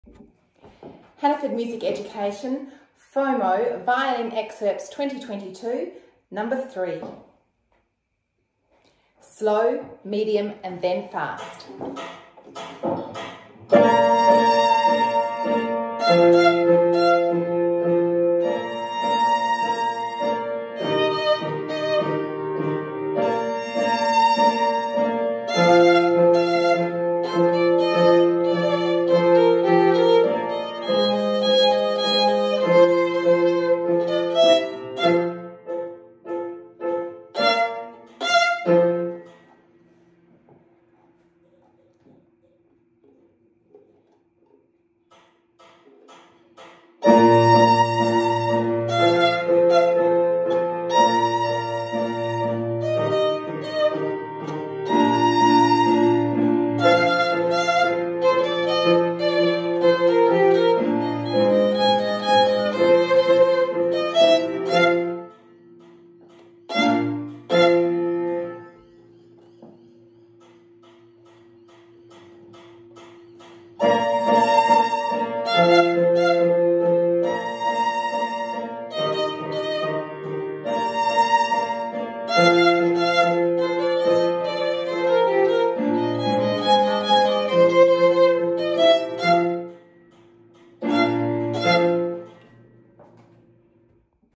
FOMO violin excerpt number 3
Festival of music Orchestra
South Australia

Violin excerpt 2022 for audition practice

Number 3 at slow, medium and fast tempo’s with accompaniment